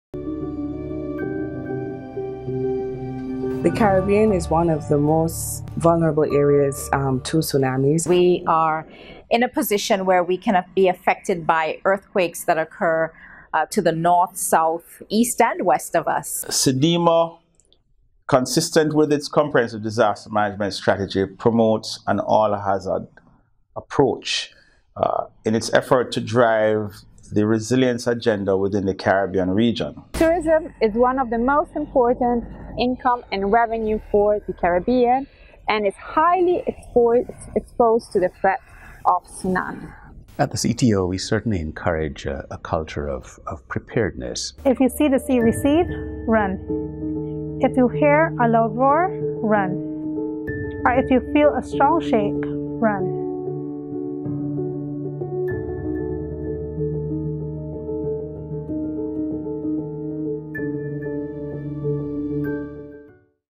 Día Mundial de Concienciación sobre Tsunamis (WTAD 2018) - Subtítulos en español - Corto
Este año, 2018, la conmemoración del Día Mundial de Concienciación sobre Tsunamis alínea con la Campaña Sendai Siete y se centra en la Meta C del Marco de Sendai para la Reducción del Riesgo de Desatres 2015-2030: Reducir las pérdidas económicas causadas directamente por los desastres en relación con el producto interno bruto (PIB) mundial para 2030. Así mismo, se enfoca en el sector turístico.